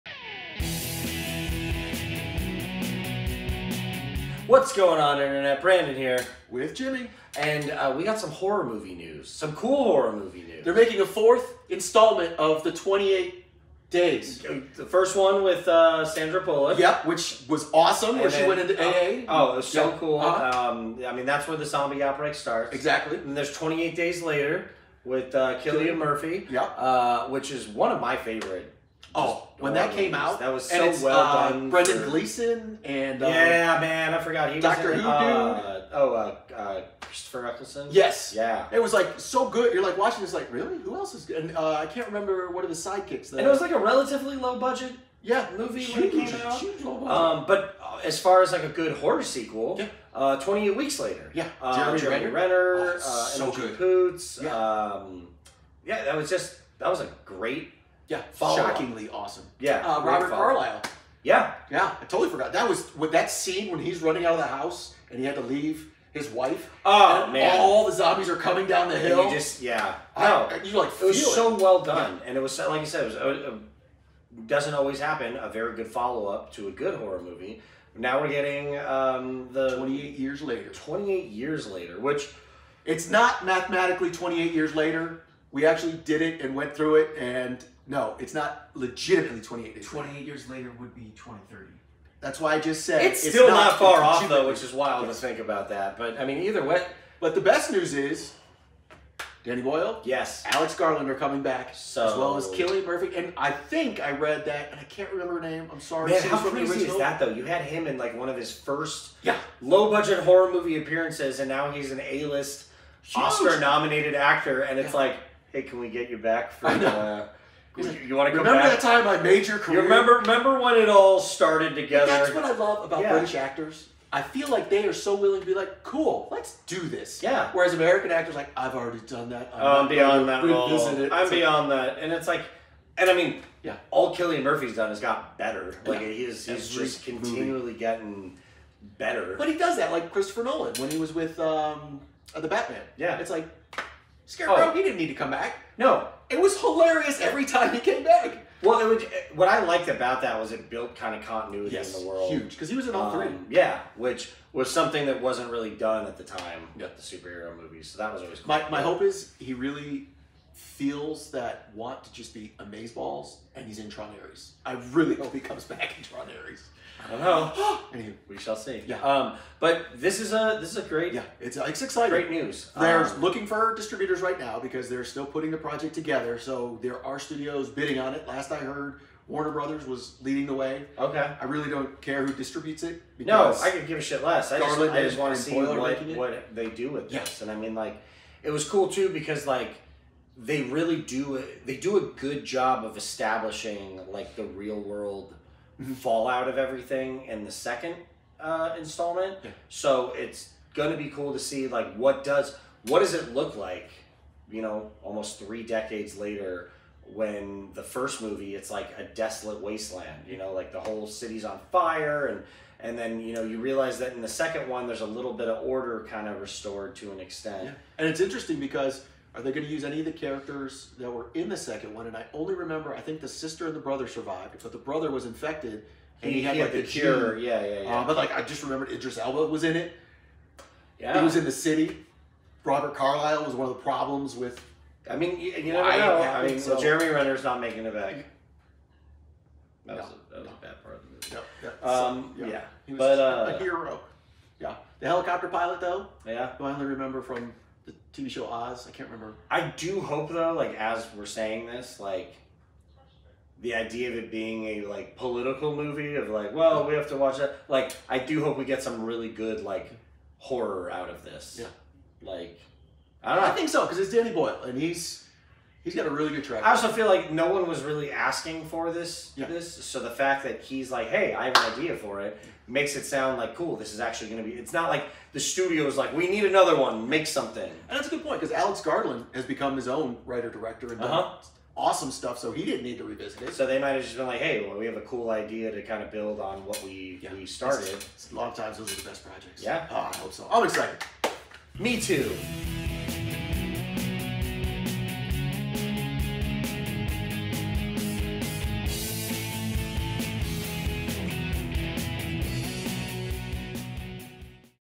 28 Years Later - We're Still Raging About Zombies!
28 Years Later - We're Still Raging About Zombies! Jimmy and Brandon discuss the news that Danny Boyle and Alex Garland are returning to the franchise they started!